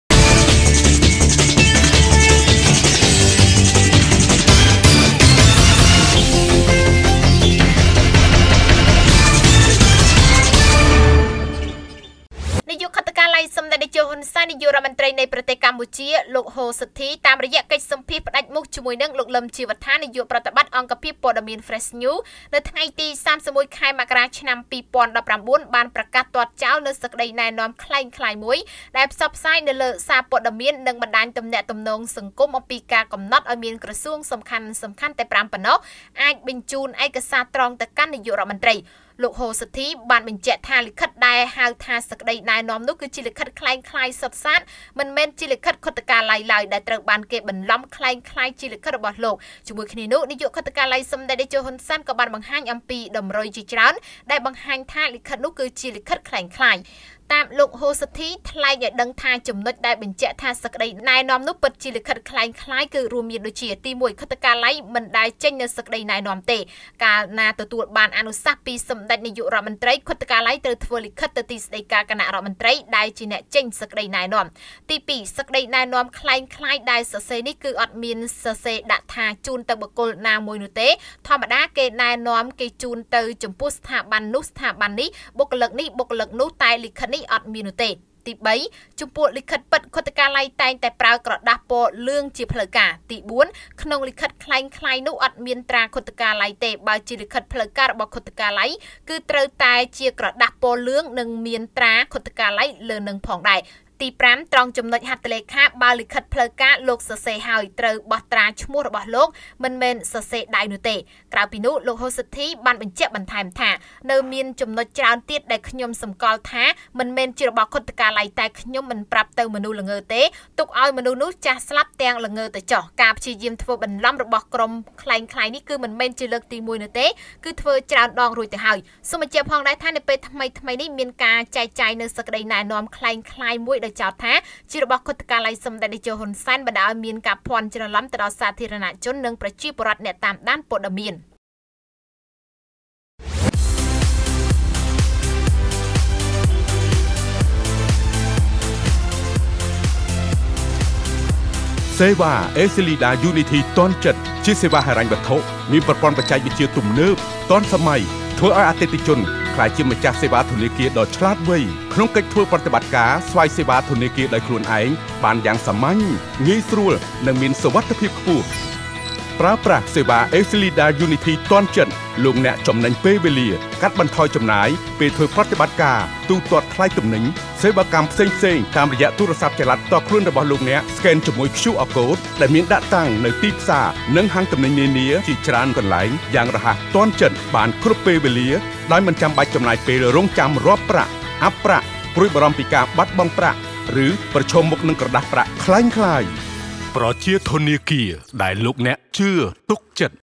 ខុទ្ទកាល័យសម្តេចតេជោ ហ៊ុន សែន ប្រកាសបដិសេធសេចក្តីណែនាំក្លែងក្លាយមួយ ...
ខុទ្ទកាល័យសម្តេចតេជោ ហ៊ុន សែន ប្រកាសបដិសេធសេចក្តីណែនាំក្លែងក្លាយមួយ ដែលកំណត់ឱ្យក្រសួង៥ បញ្ជូនឯកសារត្រង់ទៅនាយករដ្ឋមន្រ្តី
* រាយការណ៍ព័ត៌មានដោយកញ្ញា ឆេង សូរិយា, ទូរទស្សន៍ Fresh News